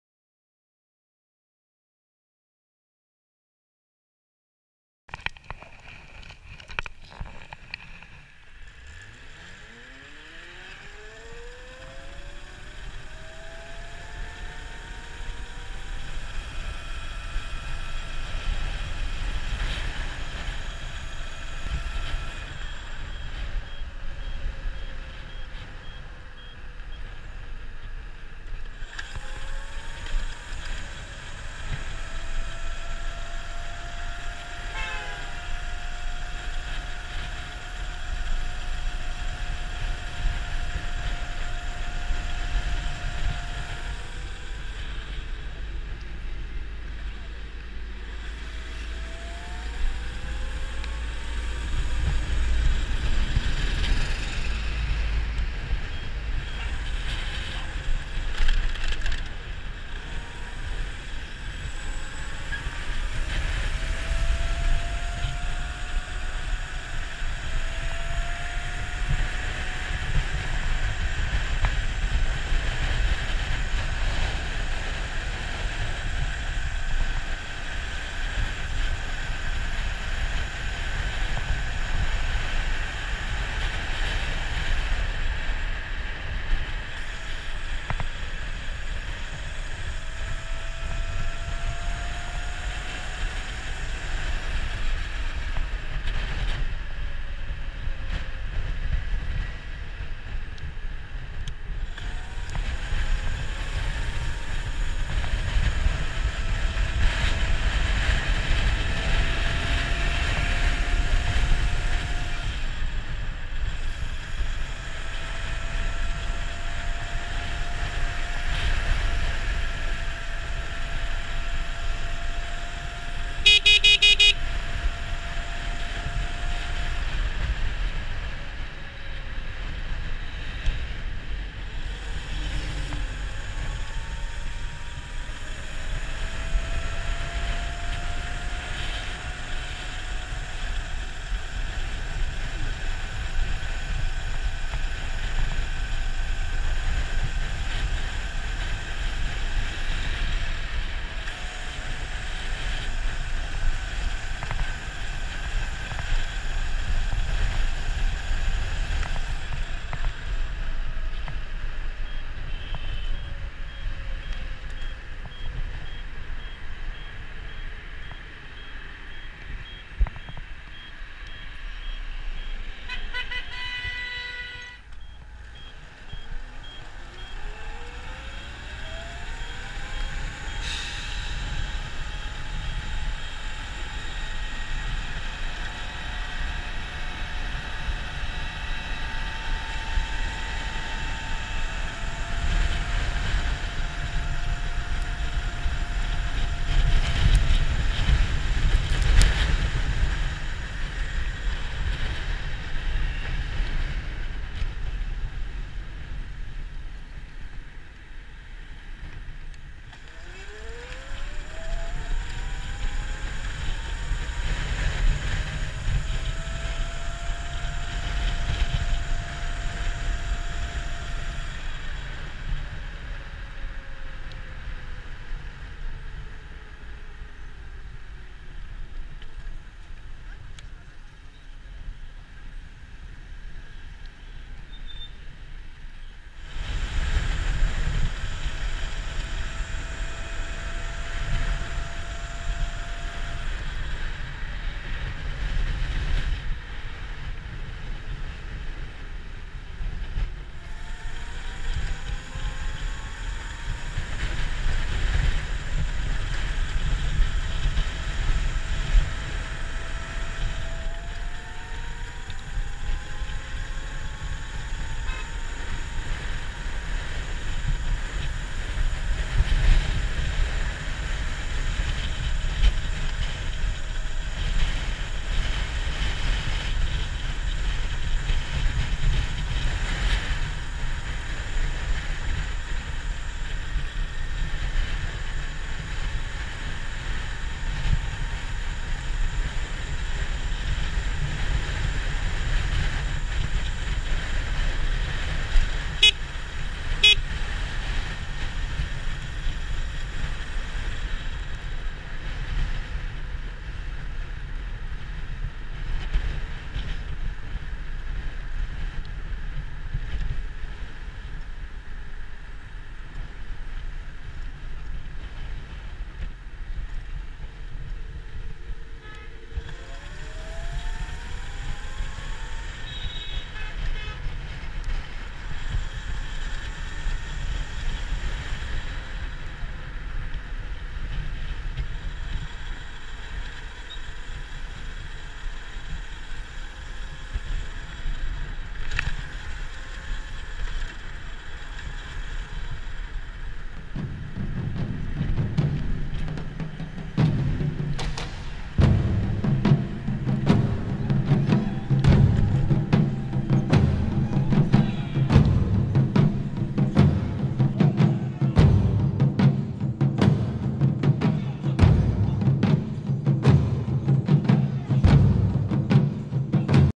Carbon Fiber Electric bike BOXON MID DRIVE
Test new battery from denzel 60V 30Ah, new BMS from denzel with bluetooth, By phone can control Battery voltage, Cells voltage, Current and many many other usefull parameters.
In next our video we introduce DENZEL SMART BMS